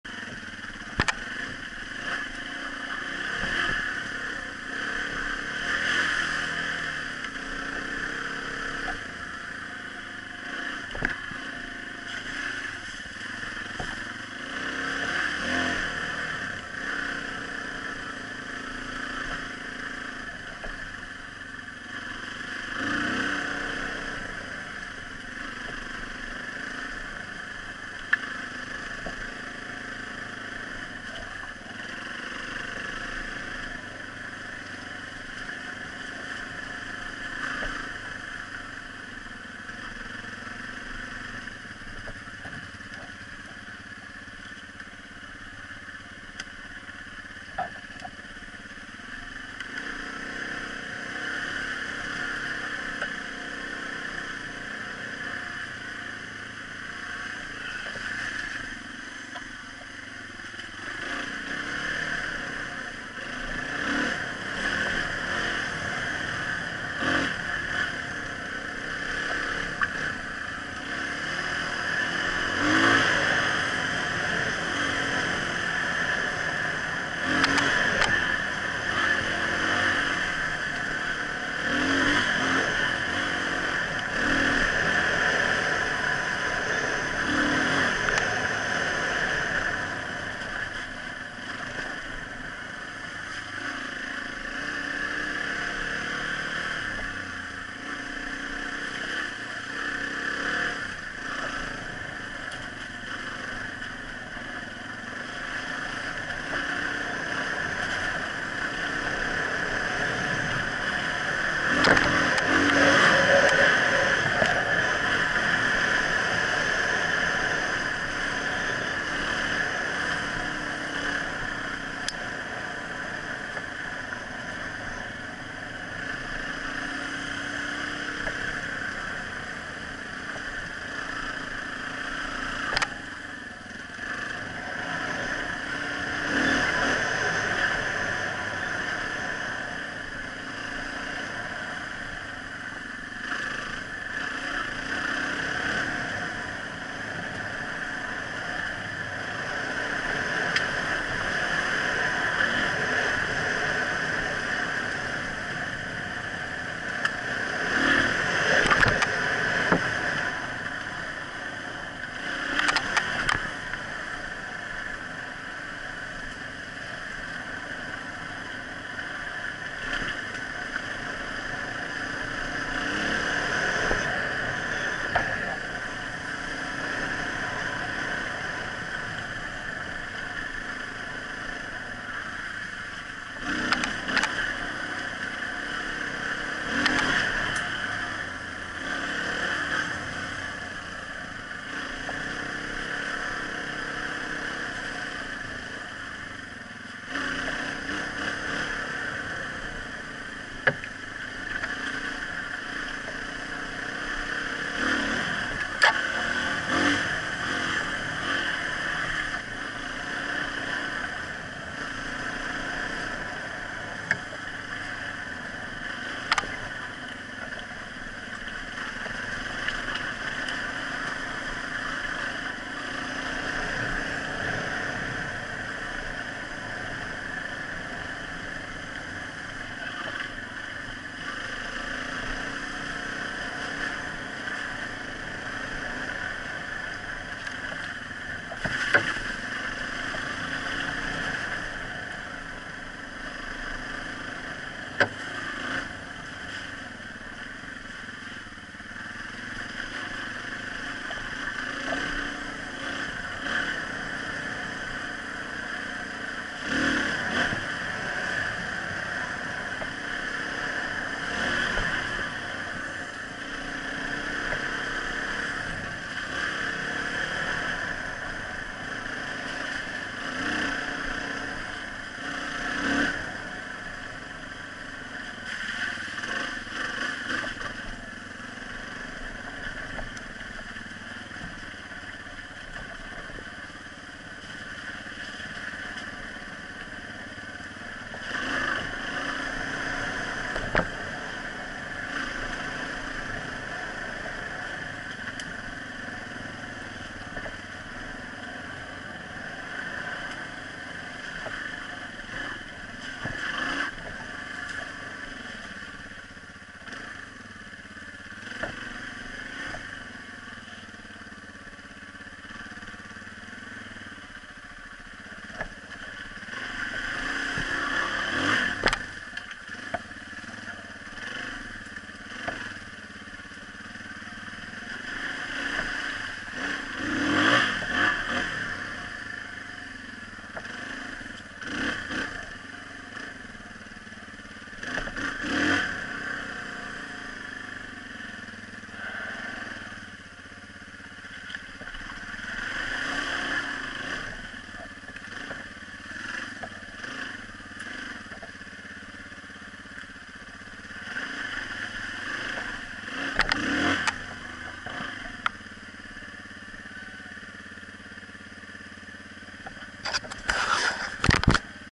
DRZ250 woods ride @ Flint Creek 1-28-16 P2
Muddy day at Flint Creek ORV trail
Bankhead National Forest